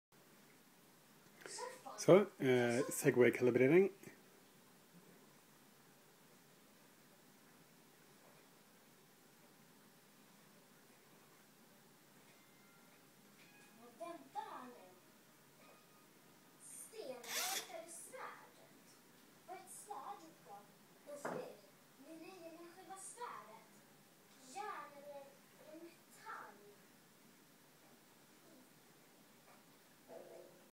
Barbie-segway calibration
Calibration of the (computer scientist) barbie segway model parameters - which will be used in the MBD course. One 40ms burst while recording gyro and currents using simulink.